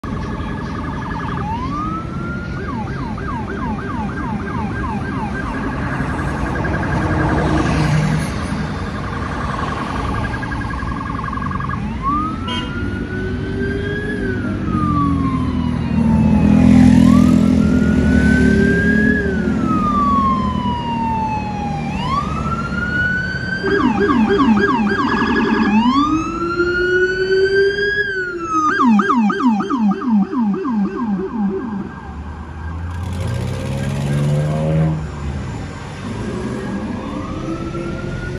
FDNY RESCUE OPERATIONS BATTALION CHIEF RESPONDING ON WEST 125TH STREET IN HARLEM, MANHATTAN, NYC.
HERE YOU WILL SEE THE FDNY RESCUE OPERATIONS BATTALION CHIEF RESPONDING ON WEST 125TH STREET IN THE HARLEM AREA OF MANHATTAN IN NEW YORK CITY.